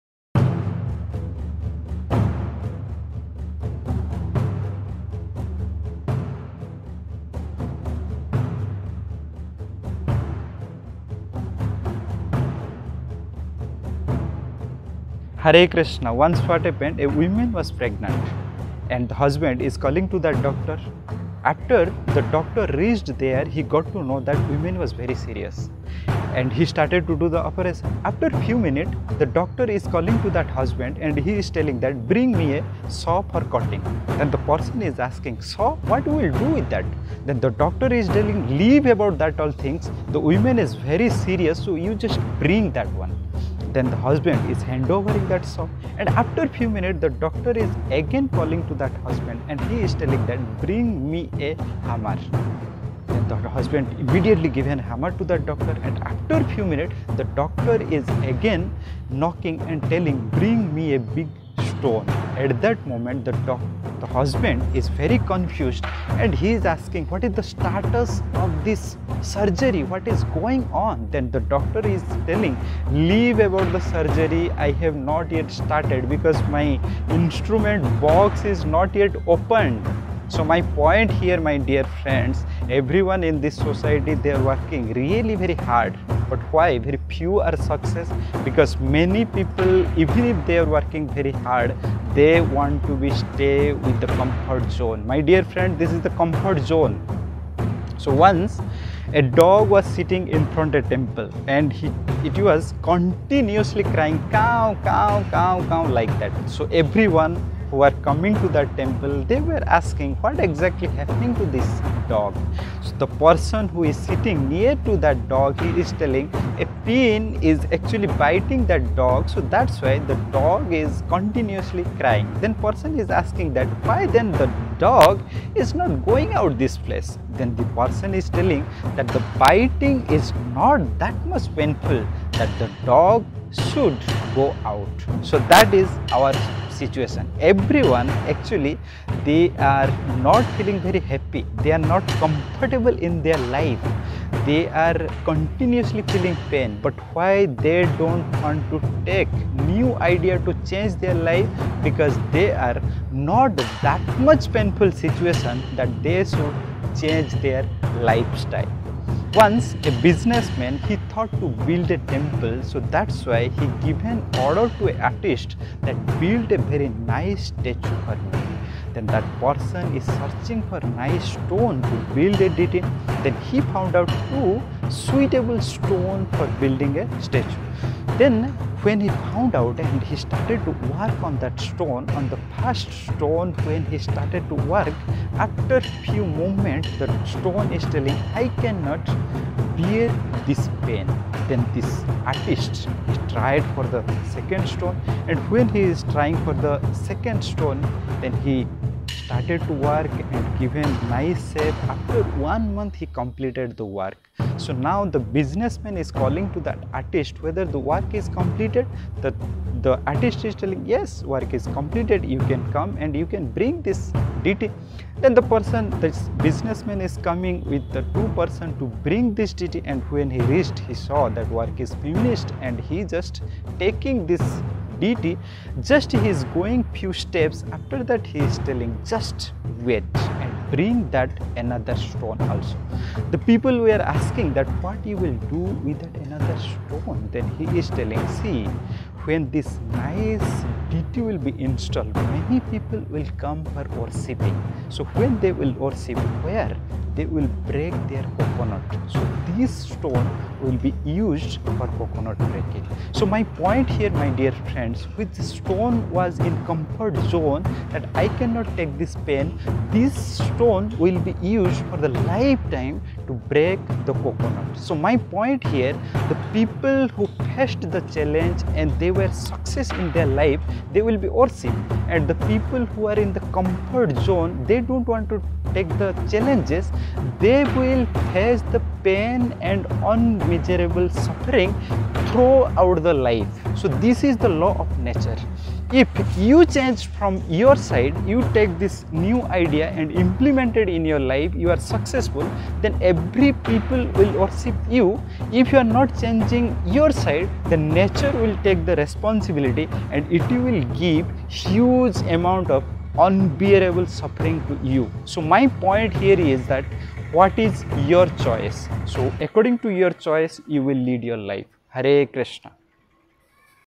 You might be in Danger, Check immediately | Motivational Video
Motivational Video how we should be proactive always so the danger can't harm us.